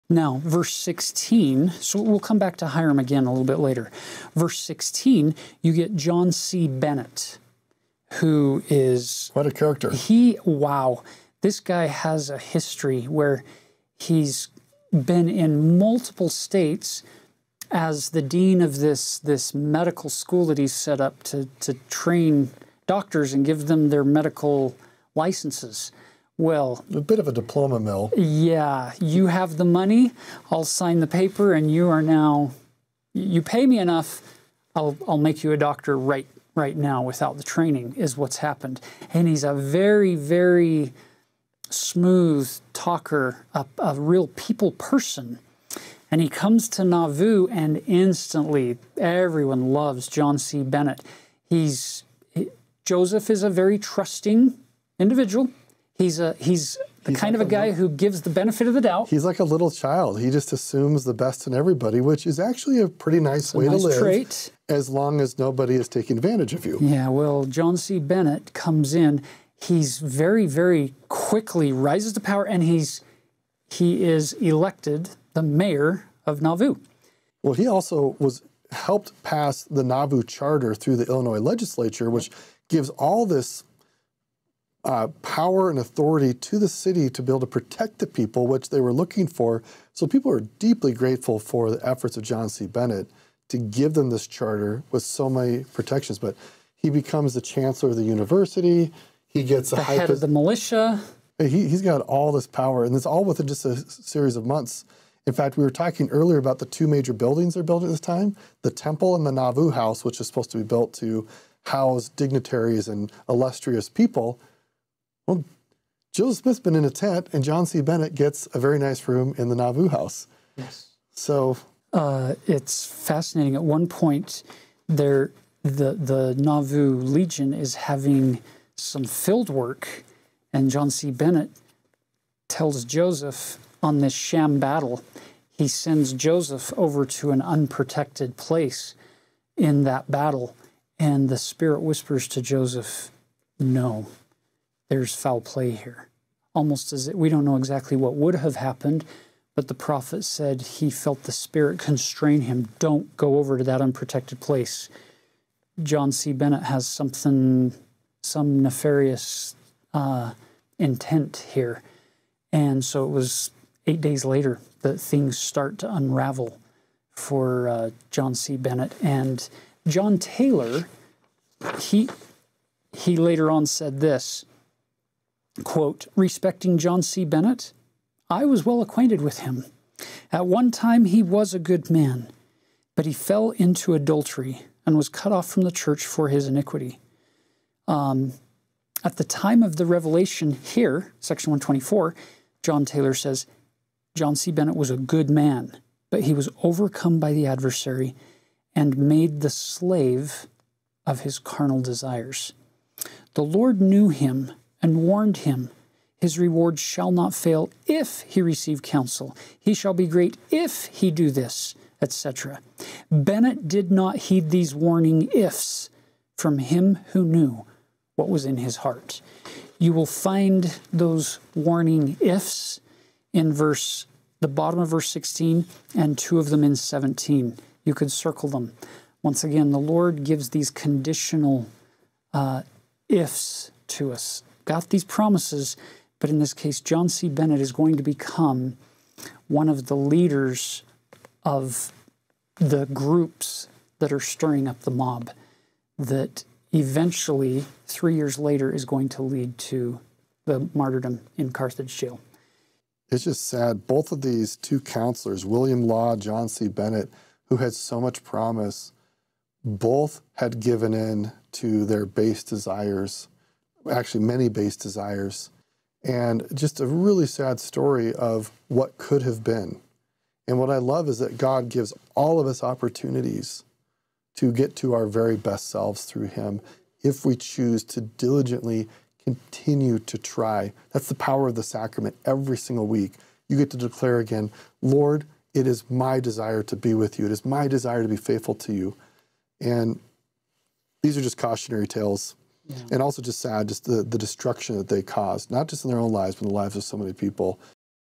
Story of John C. Bennett
Tyler Griffin and Taylor Halverson give the story of John C. Bennett.